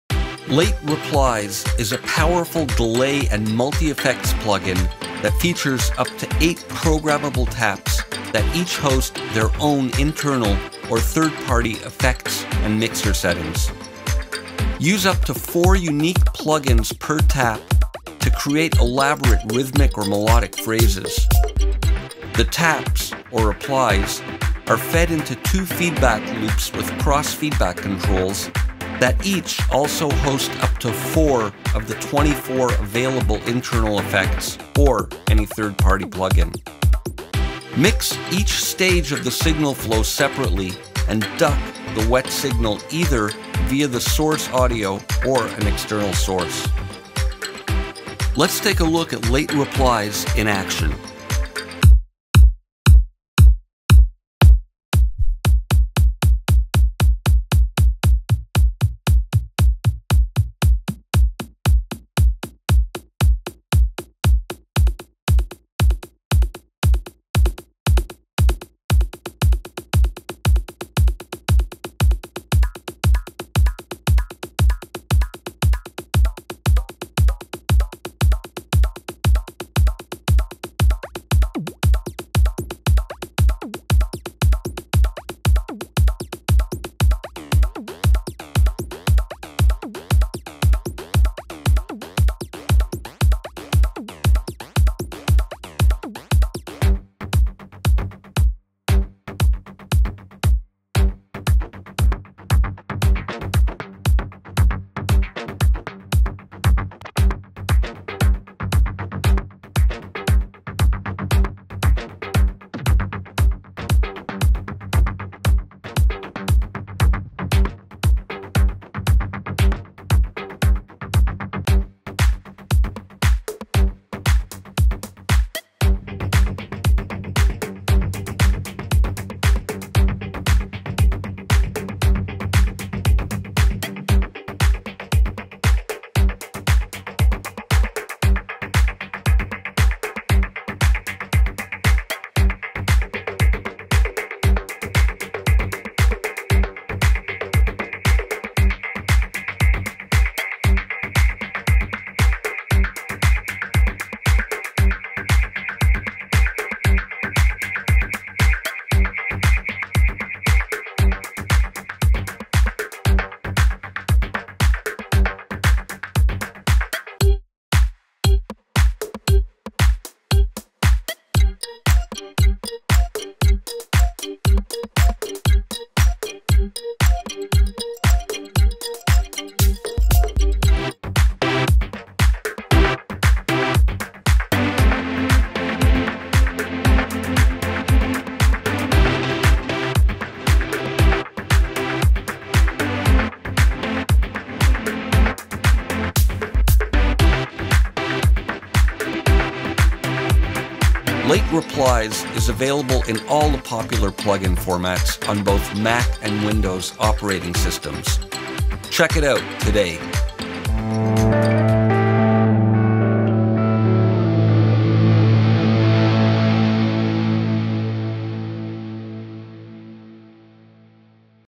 Blue Cat's Late Replies Creative Delay & Reverb Overview
Overview of Blue Cat's Late Replies, a powerful delay, reverb and multi effects plug-in that can be indefinitely extended by hosting third party VST, VST3 or AU plug-ins anywhere in the signal path.

0:00 Intro
0:15 Pattern Section
0:25 Feedback Loops
0:38 Ducking
0:47 Signal Chain
0:52 Creating Delays with FX
2:44 Melodic Patterns
3:00 Reverbs
3:21 Availability
3:31 Outro